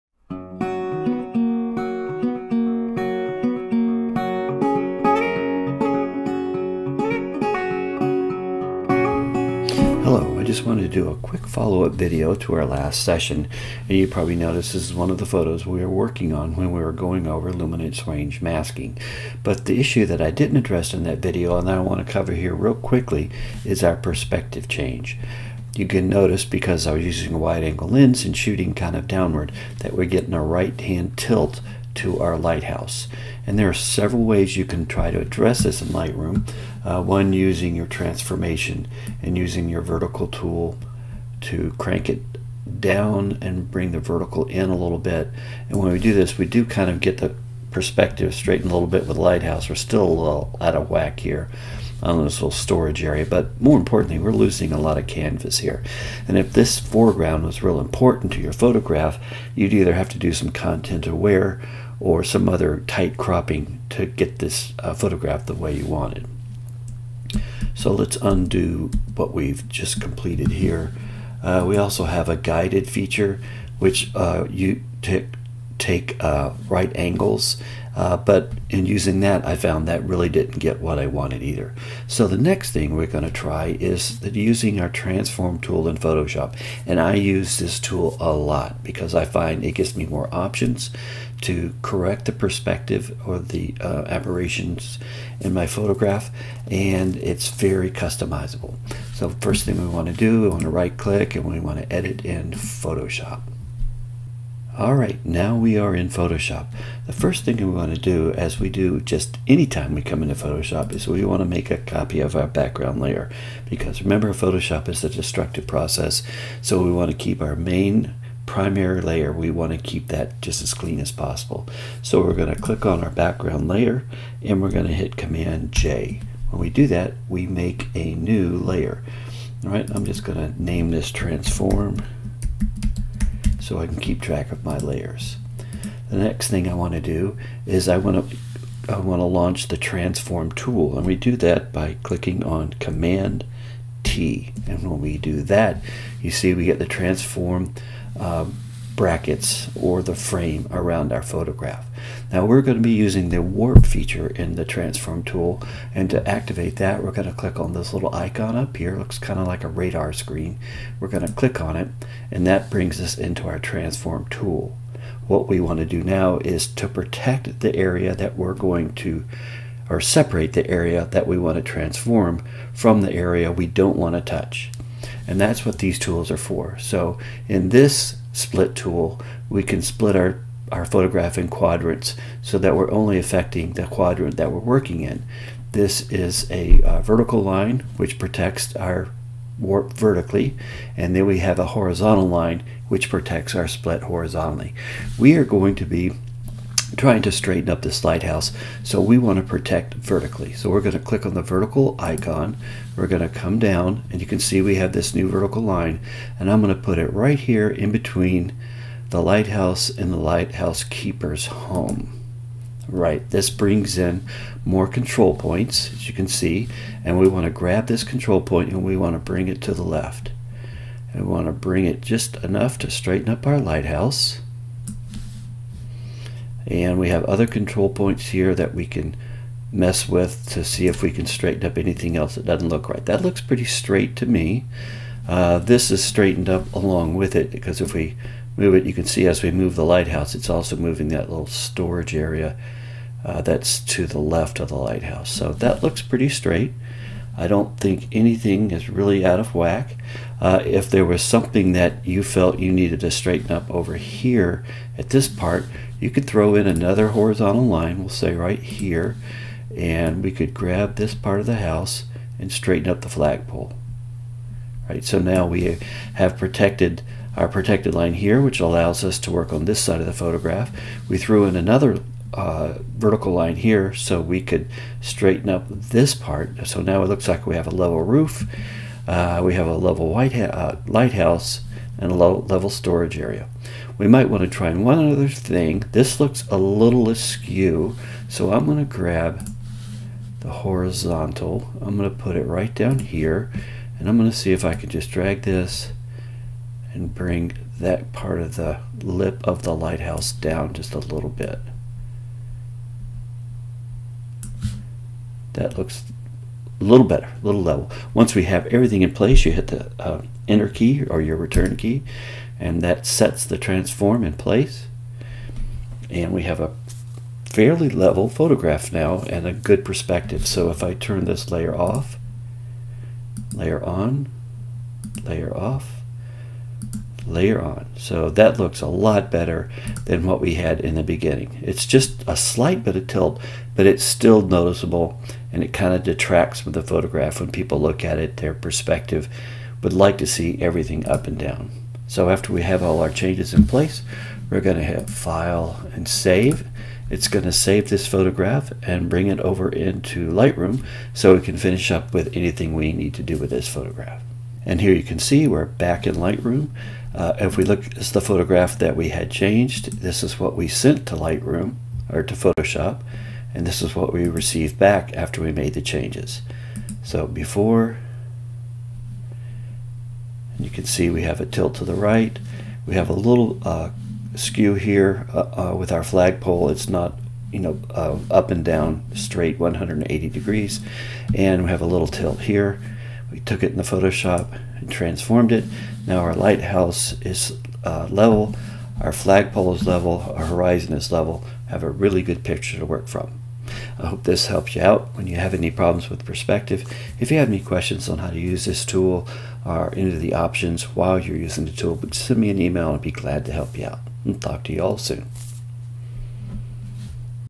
Correcting Wide Angle Lens Distortion using Lightroom and Photoshop
I review the methods we can correct  wide angle lens distortion. I concentrate on the use of the transformation tool in Photoshop to get the most customizable and effective results in correcting wide angle lens distortion.